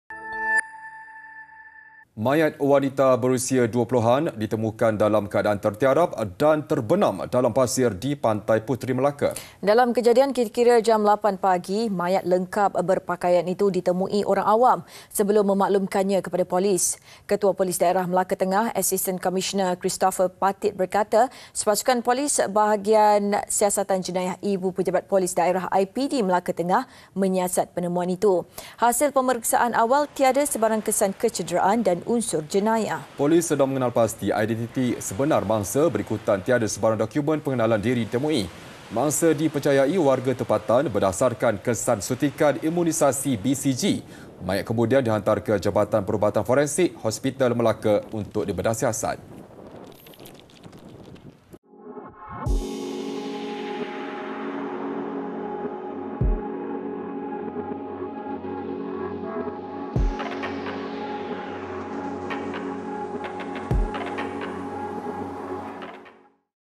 LEMAS | Mayat Wanita Terkambus Pasir Ditemukan Di Pantai Puteri
Mayat wanita berusia 20-an ditemukan dalam keadaan tertiarap dan terbenam dalam pasir di Pantai Puteri, Melaka.